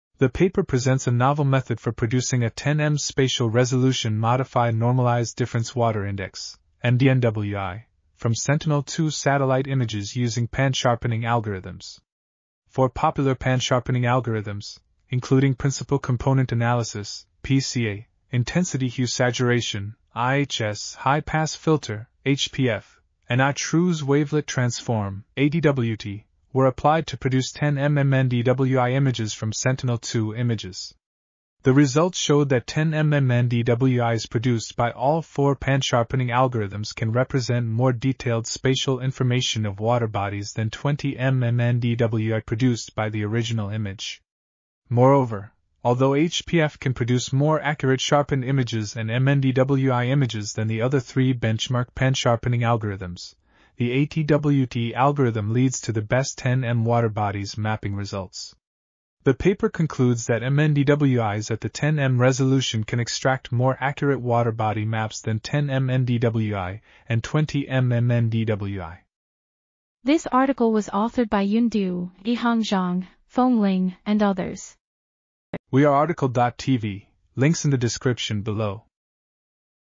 Water Bodies’ Mapping from Sentinel-2 Imagery with Modified Normalized Difference Wat... | RTCL.TV
### Keywords ###

### Article Attribution ###
Title: Water Bodies’ Mapping from Sentinel-2 Imagery with Modified Normalized Difference Water Index at 10-m Spatial Resolution Produced by Sharpening the SWIR Band
Authors: Yun Du, Yihang Zhang, Feng Ling, Qunming Wang, Wenbo Li ,and Xiaodong Li

### Video Timestamps ###
0:00:00 - Summary
0:01:09 - Title
0:01:15 - Tail
0:01:19 - End